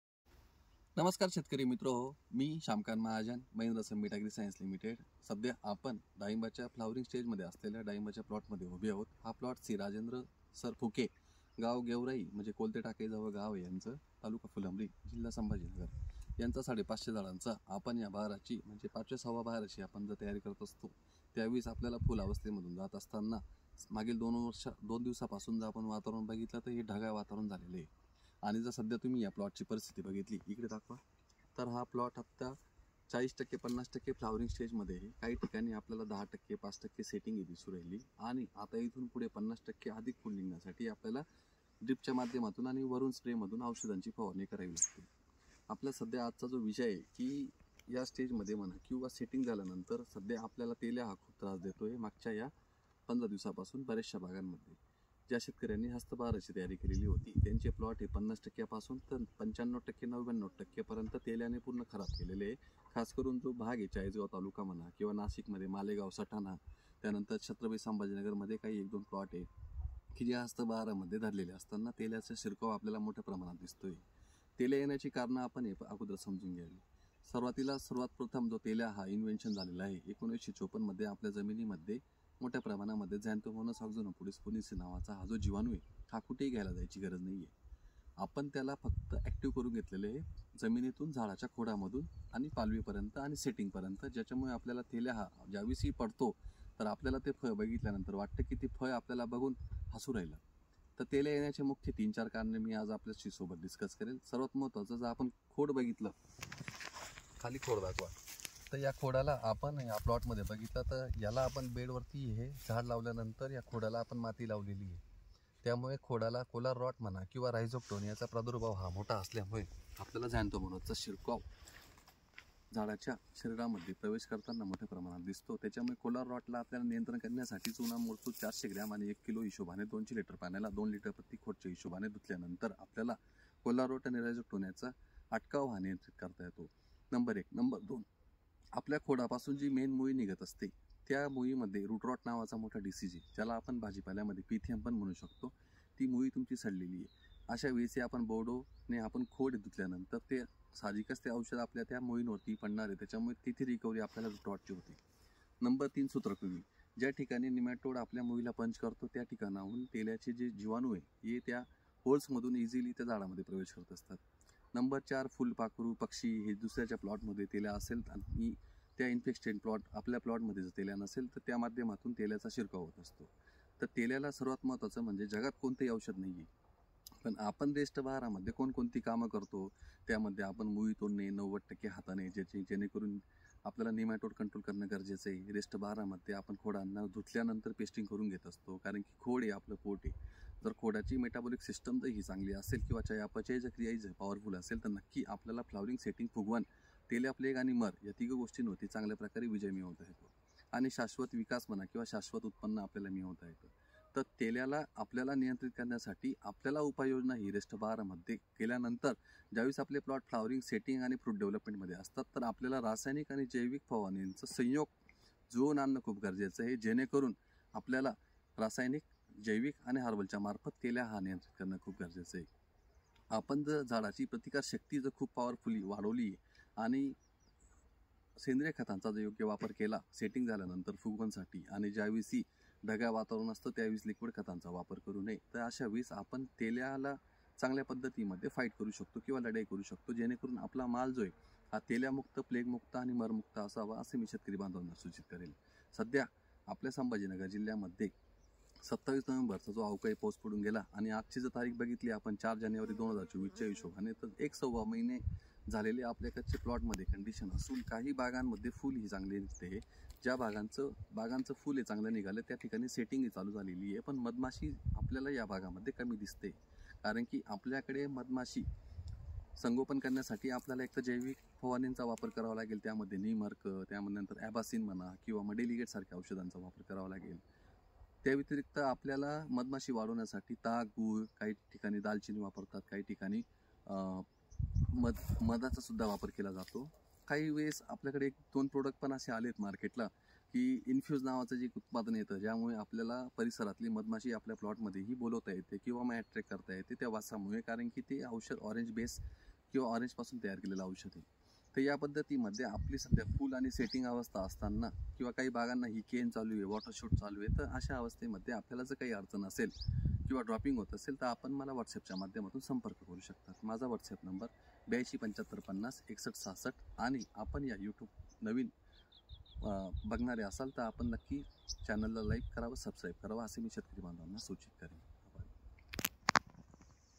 तेल्या ची मुख्य कारणे व उपाय योजना!आंबिया बहर मधील फ्लोअरिंग समस्या विवरण!*शामकांत महाजन!!!
🙋‍♂️🙋‍♂️🙋‍♂️प्रिय शेतकरी मित्र,

आज प्लॉट भेट प्रसंगी श्री राजेन्द्र भानुदास फुके,गाव - गुंगी गेवराई,फुलंब्री,छ संभाजी नगर,झाड संख्या - ५५०,२७ नोव्हेंबर आंबिया बहार पानगळ!

*मुख्य समस्या - तेल्या!!*

तेल्या या रोग आपल्याला समजून घ्यावा लागणार आहे,तो कश्यानेही जाणार नसून. *हायड्रोजन पेरॉक्साईड* च वापर सरस केला जातो आहे,पण तेल्या हा फक्त प्रतिकारशक्ती च वाचवू शकते.

*तेल्या बद्दल संक्षिप्त प्रबोधन* करण्याचा प्रयत्न हा केला असून. आपल्याला शाश्वत उपाय योजना व बोर्ड मिश्रण च जास्तीत जास्त वापर हे केलेस नक्की फायदा होतो. असे मला वाटते!

आपण हा *लेख व व्हिडिओ आवडल्यास नक्की लाईक सबस्क्राईब* करावे असे मी सूचित करेन!!!

शामकांत महाजन
महिंद्रा समिट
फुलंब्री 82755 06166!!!